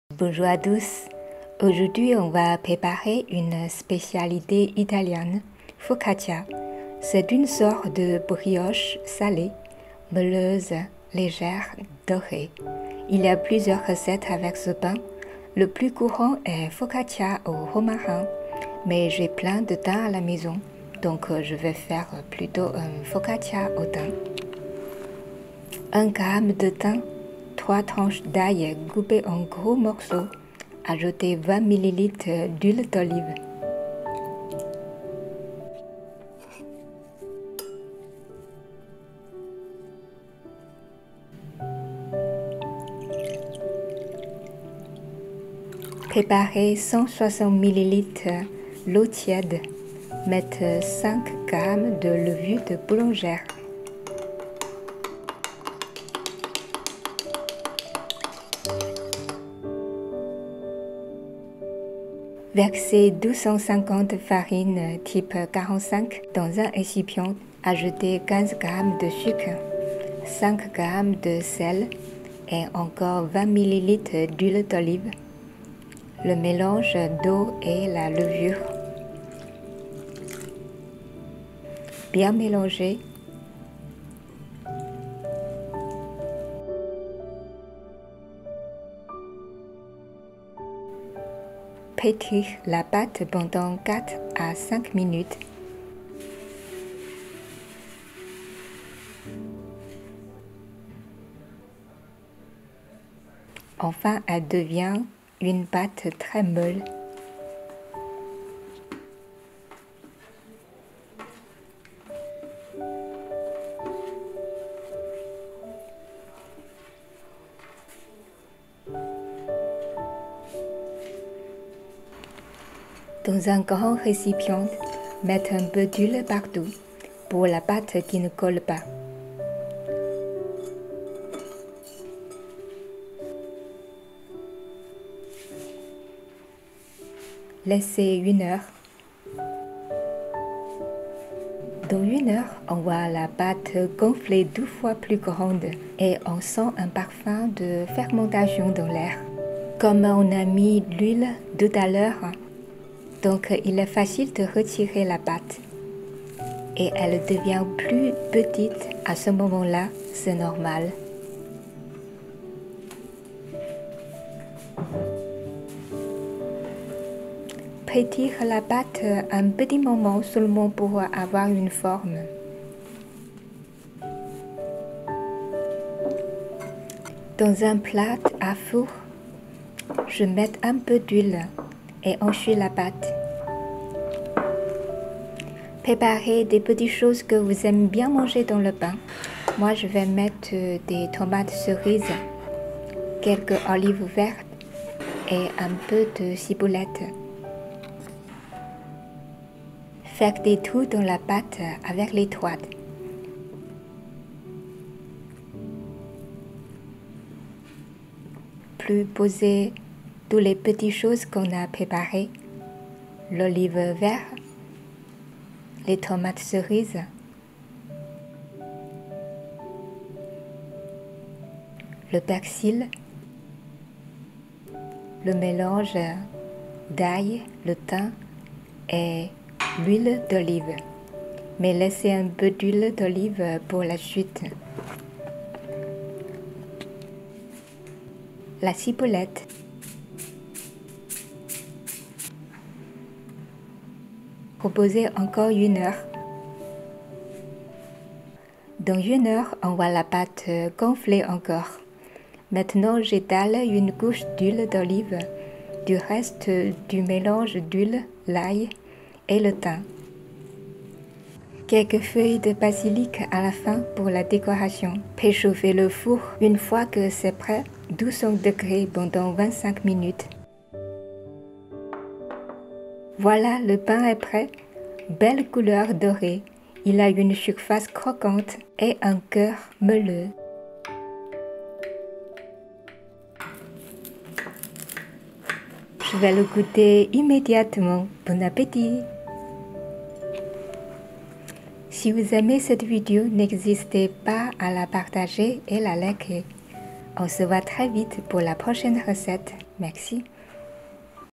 Foccacia - Pain italien
Ingrédient :
1 g de thym
3 tranches d'ail
20 ml + 20 ml d'huile d'olive
5 g de levure de boulangère
250 g de farine T45
15 g de sucre
5 g + 2 g de sel
2 ou 3 tomate cerise
Quelques olives
Un peu de ciboulette et de persil
Quelques feuilles de basilic